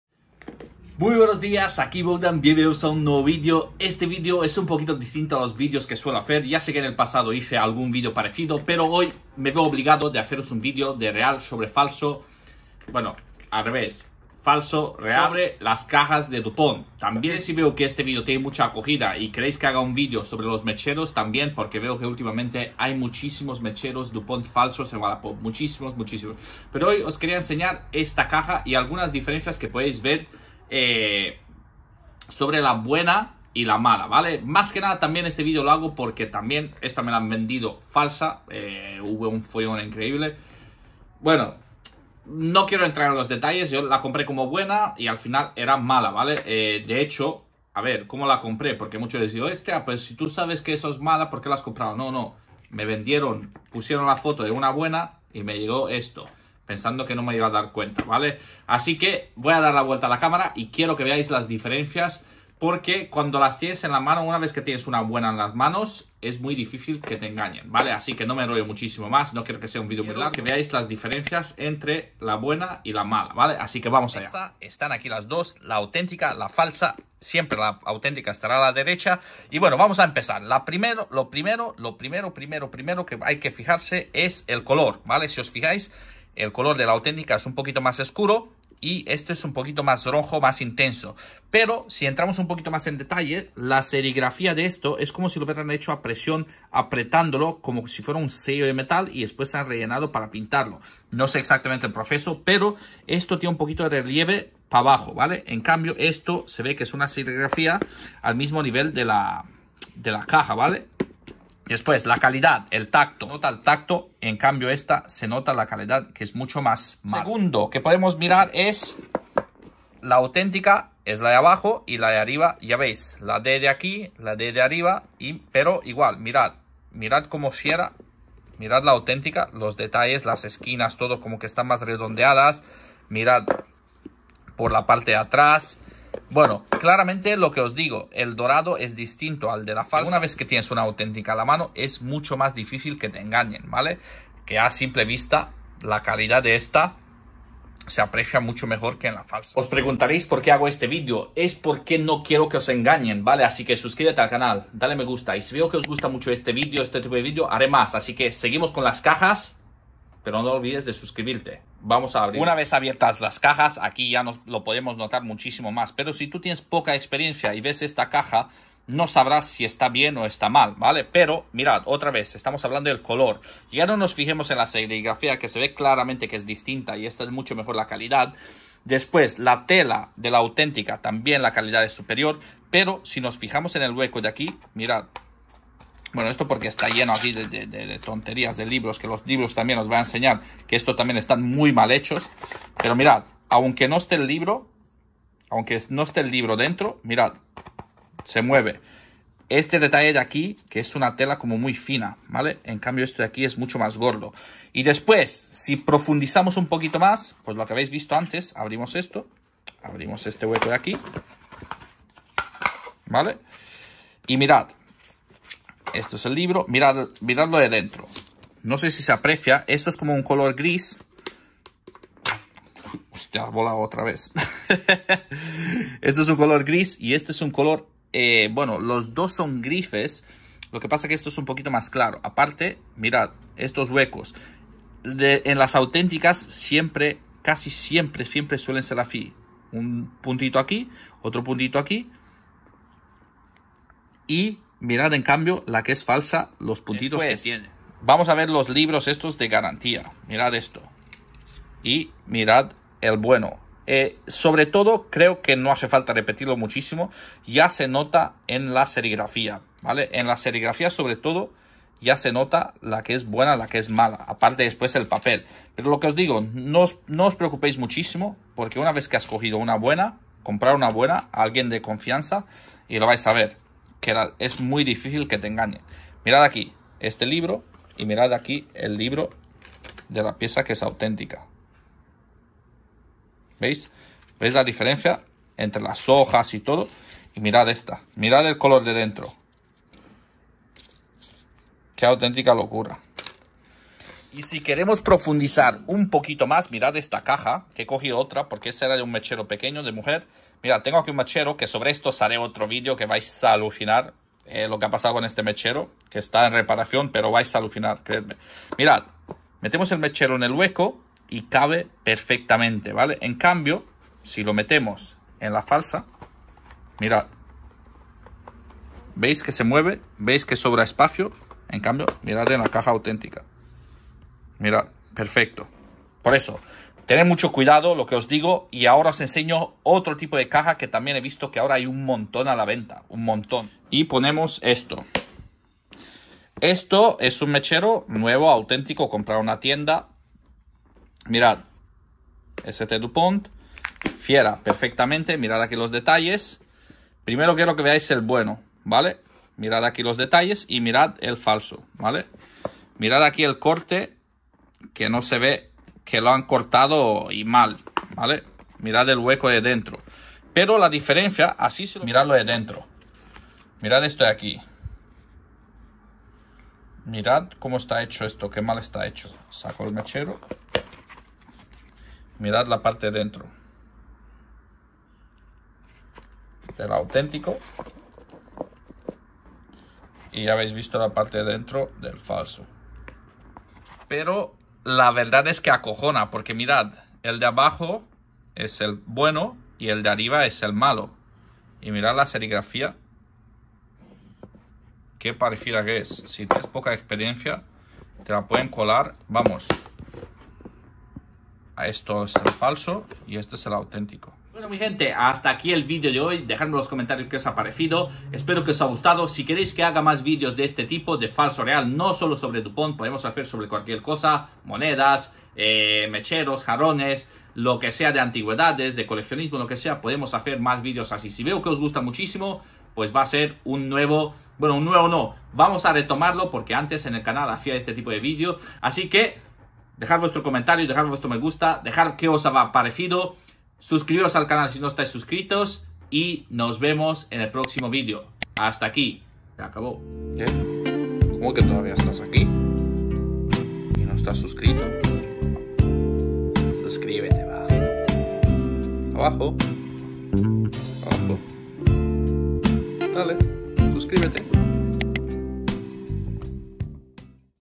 🔥Tutorial ST Dupont Paris COMO SABER SI es auténtica la Caja des Encendedor 🤔Real vs Fake 📈
MIRATE ESTE VIDEO PARA QUE NO TE CUELEN UNA CAJA FALSA DUPONT PARIS !!